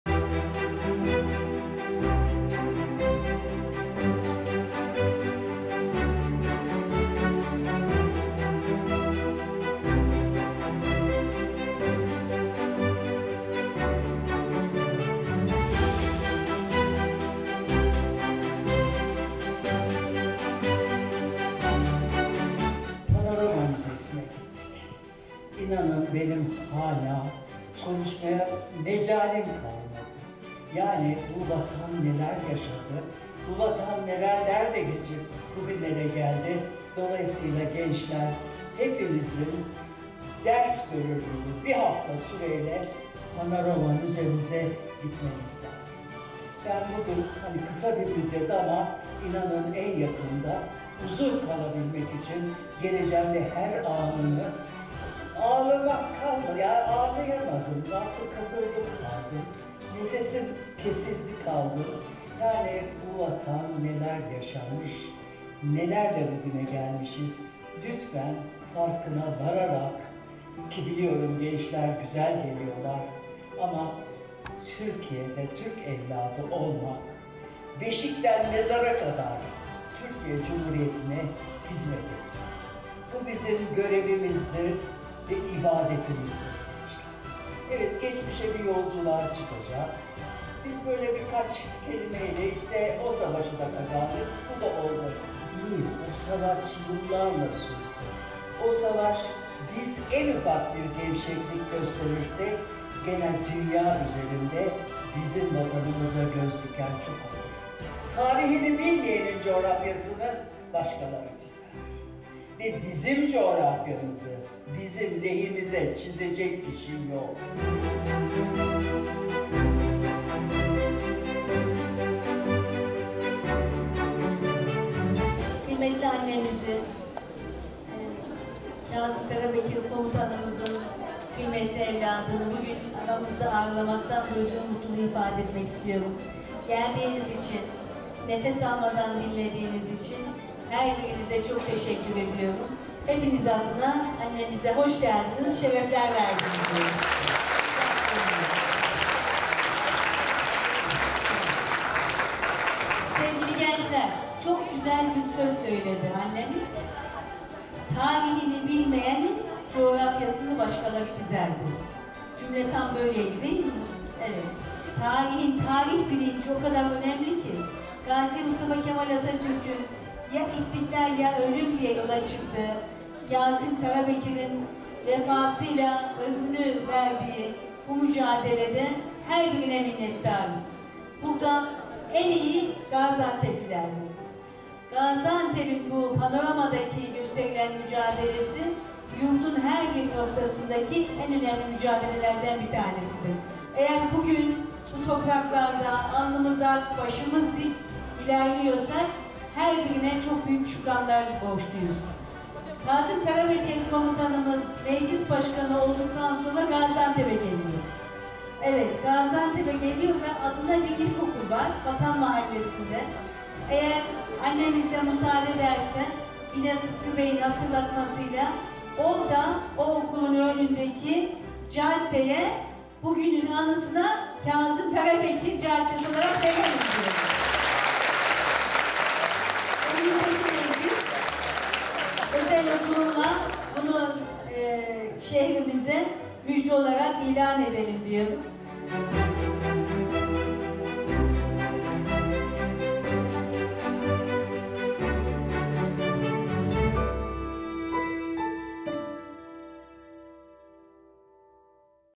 "Cumhuriyet'e Giden Yol"
Konferansta Timsal Karabekir, babası Kazım Karabekir Paşa'nın Milli Mücadele'deki rolünü ve Cumhuriyet'in kuruluş sürecini anlattı.

Konferans’ta, Milli Mücadele'nin önemi vurgulandı ve Cumhuriyet'in kuruluş sürecine ışık tutuldu.